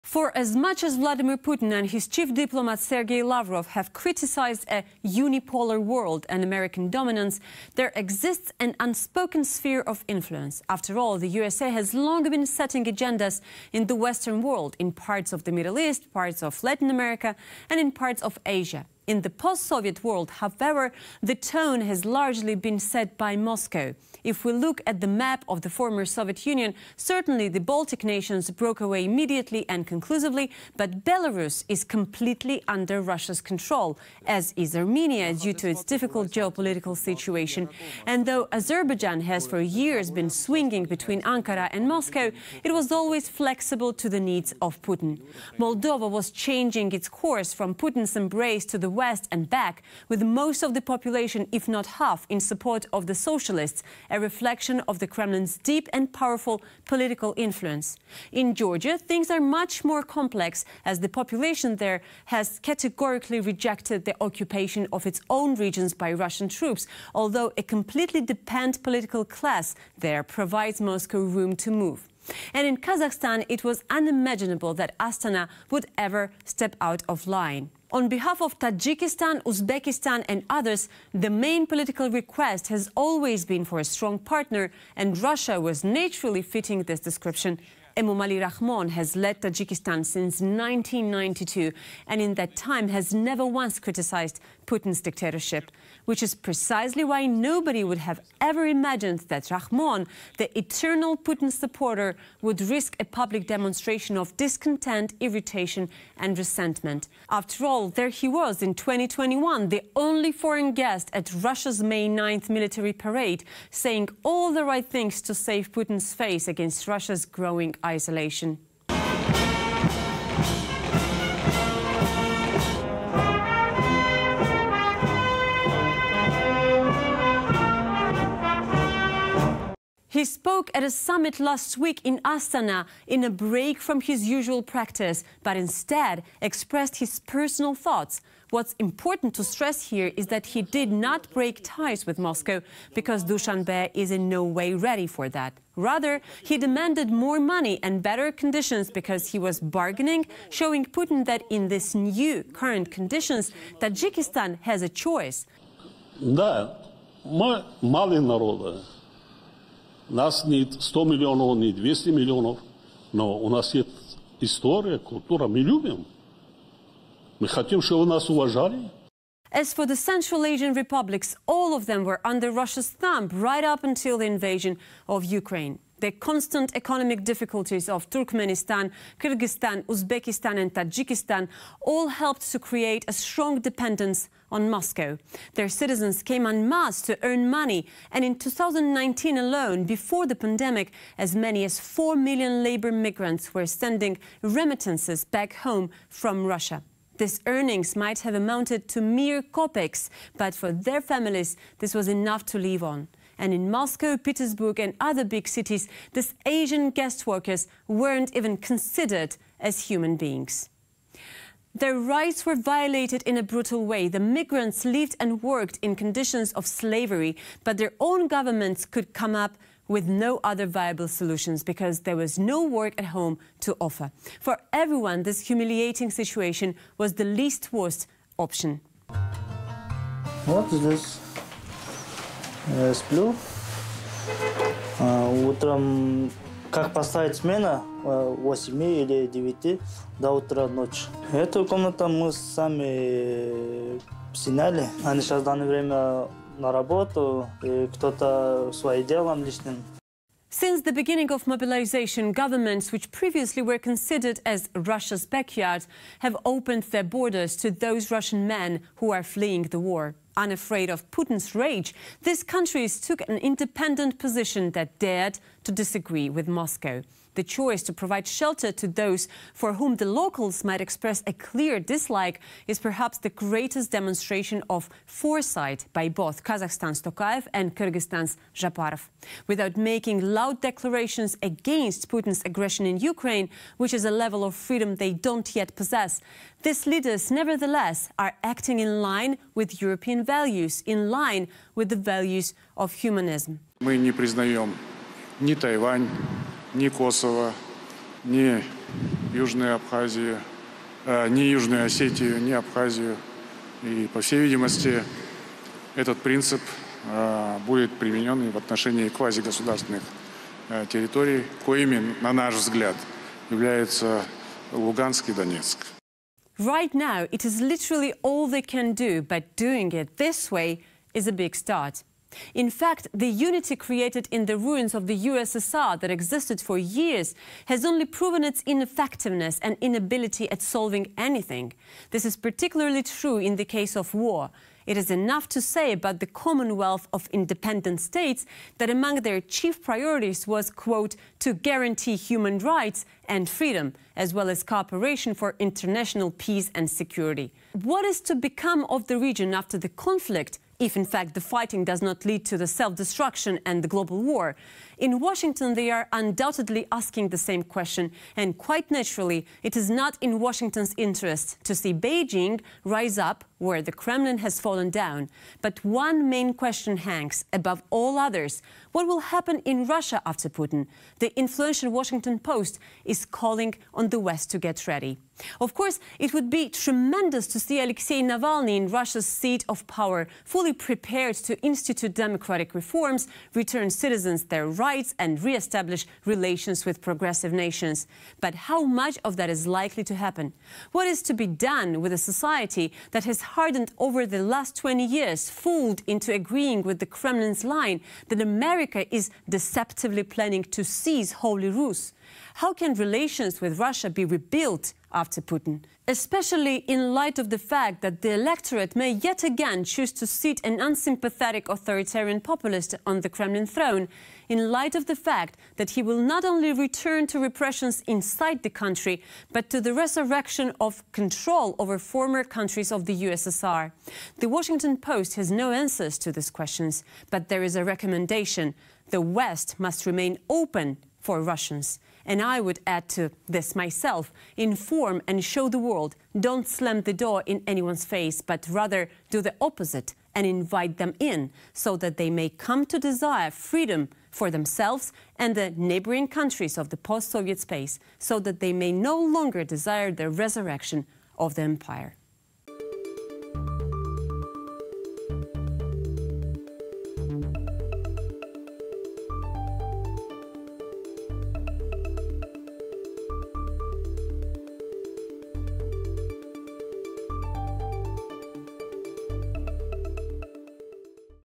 Putin's Lost Control: CIS Leader Walks The Edge In Public
How did Putin come to lose the CIS? Why do the countries within Russia's sphere of influence now abandon the Kremlin's party line and make pragmatic decisions that don't put Putin first? Ekaterina Kotrikadze looks at how Tajikistan's only post-Soviet leader dared to negotiate publicly with Russia at a recent summit in Astana. Plus more details on the mass shooting in Belgorod and how a Tajik restaurant worker went from Moscow to his death at a firing range near the Ukraine border after falling into the Russian army.